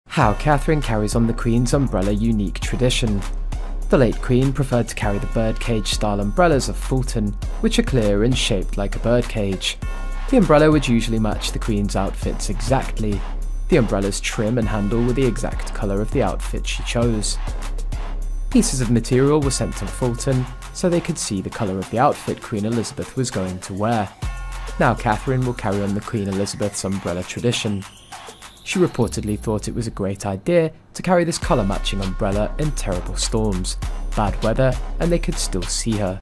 How Catherine Carry On The Queen’s Umbrella Unique Tradition
Late Queen preferred to carry the “birdcage” style umbrellas of Fulton, which are clear and shaped like a bird cage. The umbrella would usually match the queen’s outfits exactly. The umbrella’s trim and handle were the exact color of the outfit she chose.